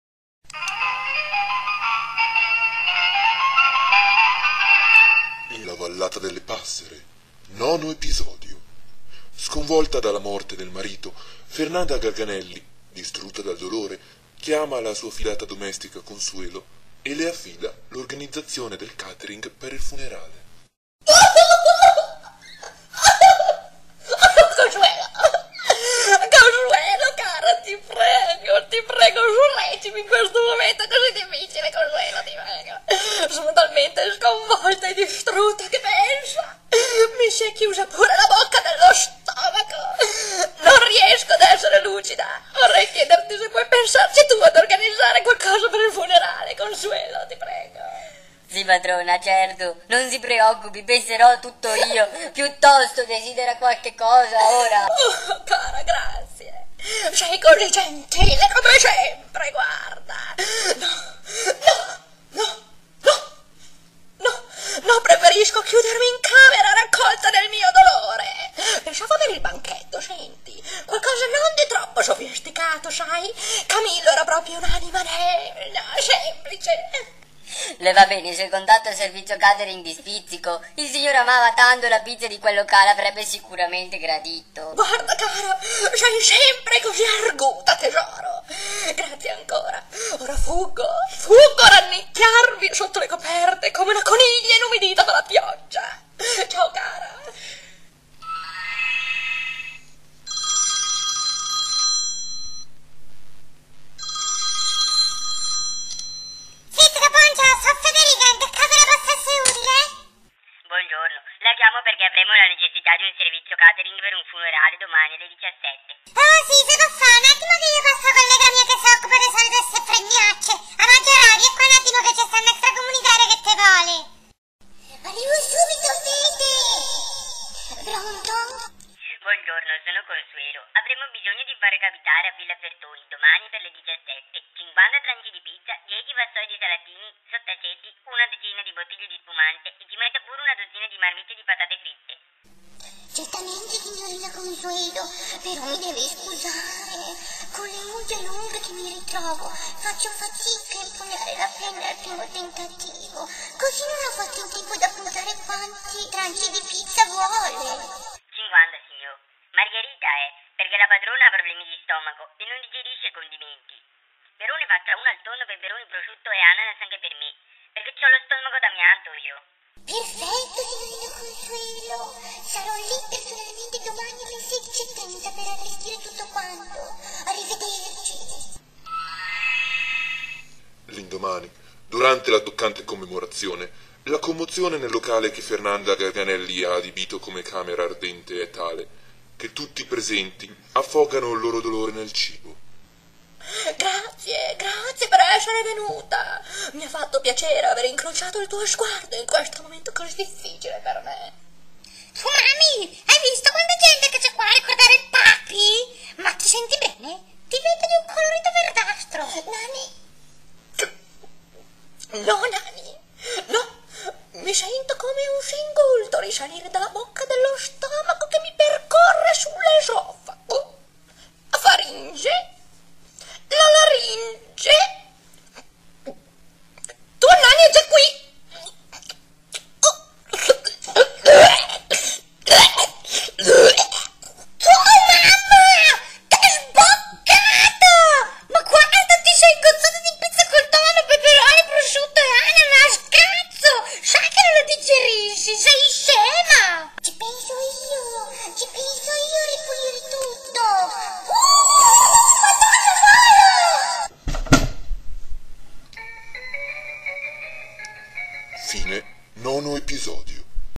La Vallata delle Passere - Episodio 9
Sconvolta dalla morte del marito, Fernanda Garganelli, distrutta dal dolore, chiama la sua fidata domestica Consuelo e le affida l'organizzazione del catering per il funerale.

Consuelo cara, ti prego, sorreggimi in questo momento così difficile. Sono talmente sconvolta e distrutta che, pensa, mi si è pure chiusa la bocca dello stomaco. Non riesco ad essere lucida, vorrei chiederti se puoi pensarci tu ad organizzare qualcosa per il funerale...
Certo signora, non si preoccupi, penserò a tutto io. Piuttosto, desidera qualcosa, ora?
Oh cara, grazie, sei così gentile. No no, preferisco chiudermi in camera, raccolta nel mio dolore... Pensavo per il banchetto, qualcosa di non troppo sofisticato, sai, Camillo era proprio un'anima semplice...
Le va bene se contatto il servizio catering di Spizzico? Il signore amava la pizza di quel locale, avrebbe sicuramente gradito...
Sei sempre così arguta, tesoro. Grazie ancora, ora fuggo a rannicchiarmi sotto le coperte come una coniglia inumidita dalla pioggia.

Spizzico buongiò, so' Federica, in cosa posso esserle utile?
Buongiorno, la chiamo perchè avremmo la necessità di un servizio catering per un funerale, domani alle 17.
Sì, se poffà, 'nattimo che je passo a collega mia che se occupa de solito de ste fregnacce... A Maggiorà, viè qua 'nattimo che ce sta 'na stracomunitaria che tte vole!
Arrivo subito Fede... Pronto?
Buongiorno, sono Consuelo, avremmo bisogno di far recapitare a Villa Fertoni, domani, per le 17, 50 tranci di pizza, 10 vassoi di salatini, sottaceti, una decina di bottiglie di spumante e ci metta pure una dozzina di marmitte di patatine fritte.
Certamente signorina Consuelo, però mi deve scusare: con le unghie lunghe che mi ritrovo faccio fatica ad impugnare la penna al primo tentativo, così non ho fatto in tempo ad appuntare quante quanti tranci di pizza vuole.
50, margherita, perchè la padrona ha problemi di stomaco e non digerisce i condimenti, però ne faccia una al tonno, peperoni, prosciutto e ananas per me, che c'ho lo stomaco d'amianto, io.
Perfetto. Sarò lì personalmente domani alle 16.30 per allestire tutto quanto. Arrivederci.

L'indomani, durante la toccante commemorazione, la commozione nel locale che Fernanda Garganelli ha adibito come camera ardente è tale che tutti i presenti affogano il loro dolore nel cibo.

Grazie per essere venuta, mi fa piacere aver incrociato il tuo sguardo in questo momento per me così difficile.
Tu mami, hai visto quanta gente che c'è a ricordare il papi? Ma... ti senti bene? Ti vedo di un colorito verdastro...
Nani, no... no... mi sento come un singulto risalire dalla bocca dello stomaco, che mi percorre l'esofago... la faringe... la laringe... tu è già qui... ... blll..blll bbeeeeeehaaaa
Tu mammaaaaaaaa! Che sboccata! Ma guarda, ti sei ingozzata di pizza con tonno, peperoni, prosciutto e ananas, cazzo! Sai che non la digerisci!

-da lontano, irrompendo sulla scena della tragedia

Ci penso io a ripulire tutto. OOOOOOpsssss! Oh Madonna volo!

Maggiorata scivola sui resti di pizza ingurgitata da Fernanda e precipita prepotentemente con il seno nella bara dove giace il corpo di Camillo Fertoni

Fine nono episodio